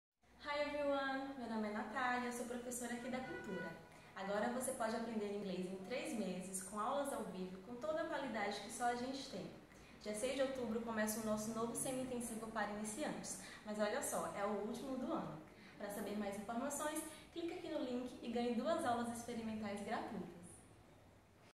Semi Intensivo - Beginner 1 (ÚLTIMA TURMA DO ANO)
🦁 Essa é a sua última chance do ano para começar a mudar sua história com a Cultura Inglesa. Semi-Intensivo Beginner 1 com material digital e preço especial 🔥. Você ainda pode fazer 2 aulas experimentais para conhecer esse super curso ao vivo, que tem toda a qualidade e certificação da Cultura Inglesa. ⠀⠀
⏱ Serão 35 horas de muito conteúdo ao vivo, toda terça e quinta-feira a noite.⠀
🗓 06/10 a 03/12 - Beginner 1 (35h)⠀
Clique no link na bio, saiba mais e garanta a sua vaga.⠀